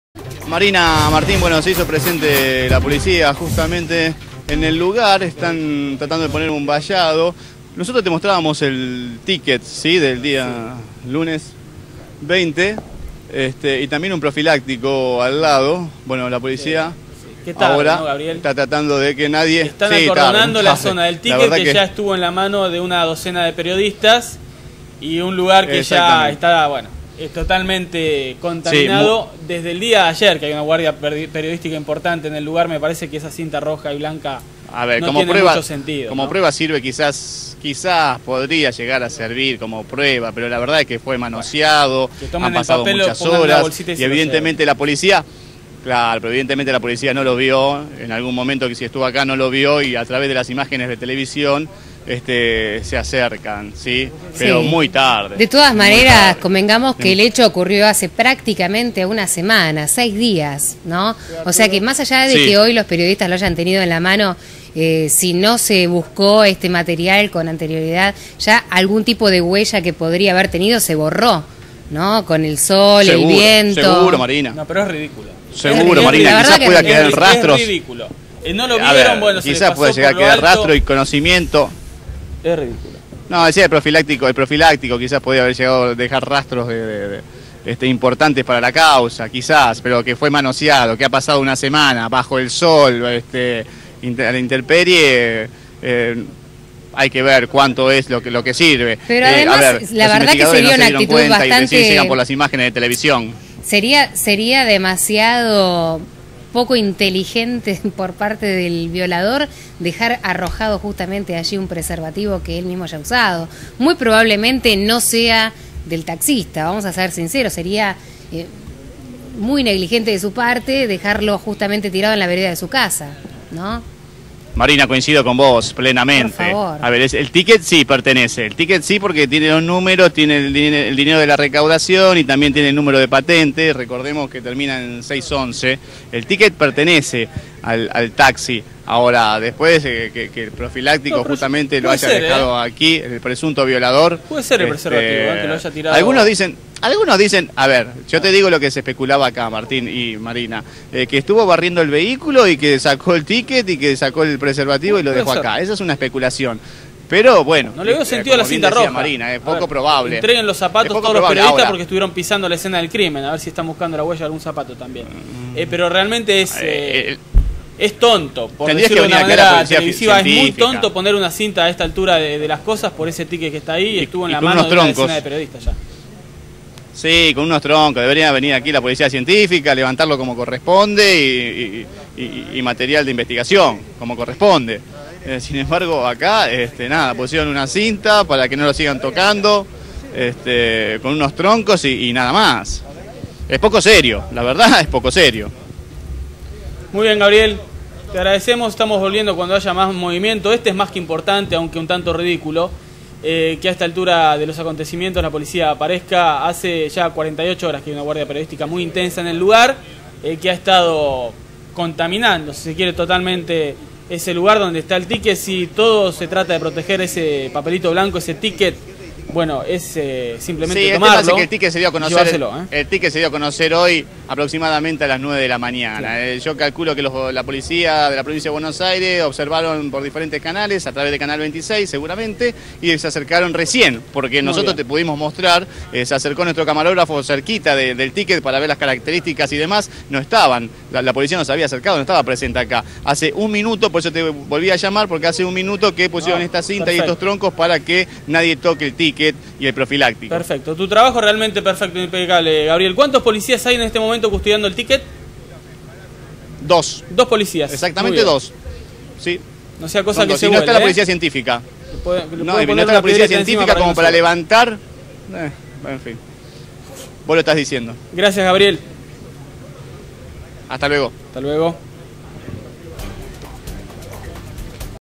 Canal 26 -Orden de detención para el taxista
La policia en la casa del presunto taxista violador